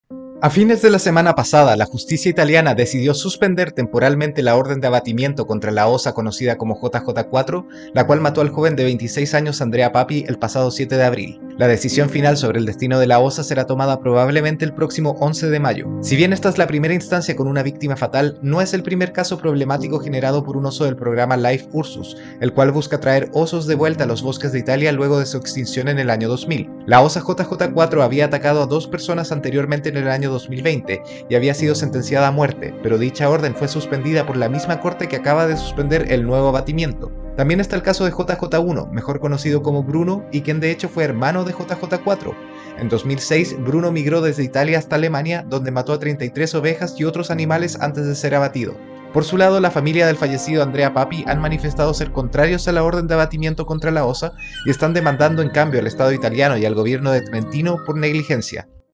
Italia no matará a la Osa JJ4 (por ahora)
A fines de la semana pasada, la justicia italiana decidió suspender temporalmente la orden de abatimiento contra la osa conocida como JJ4, la cual mató al joven de 26 años Andrea Papi el pasado 7 de abril. La decisión final sobre el destino de la osa será tomada probablemente el próximo 11 de mayo.
Si bien esta es la primera instancia con una víctima fatal, no es el primer caso problemático generado por un oso del programa Life Ursus, el cual busca traer osos de vuelta a los bosques de Italia luego de su extinción en el año 2000.
La osa JJ4 había atacado a dos personas anteriormente en el año 2020 y había sido sentenciada a muerte, pero dicha orden fue suspendida por la misma corte que acaba de suspender el nuevo abatimiento.
También está el caso de JJ1, mejor conocido como Bruno, y quien de hecho fue hermano de JJ4. En 2006, Bruno migró desde Italia hasta Alemania, donde mató a 33 ovejas y otros animales antes de ser abatido.
Por su lado, la familia del fallecido Andrea Papi han manifestado ser contrarios a la orden de abatimiento contra la osa y están demandando en cambio al estado italiano y al gobierno de Trentino por negligencia.

Conviértete en mecenas y obtén beneficios especiales y acceso a contenidos exclusivos por apenas 1$ mensual!